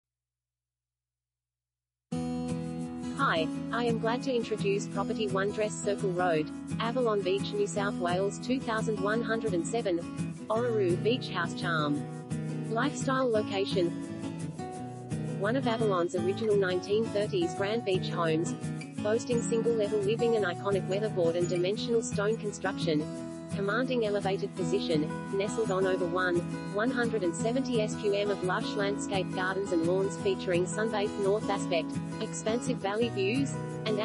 1 Dress Circle Road, Avalon Beach NSW 2107 | AuPropertyReport.Com
1 Dress Circle Road, Avalon Beach NSW 2107
Property Type: house
Bedrooms: 4
Bathrooms: 4
Car Space: 3
&quot;Orroroo&quot;  Beach House Charm! Lifestyle Location!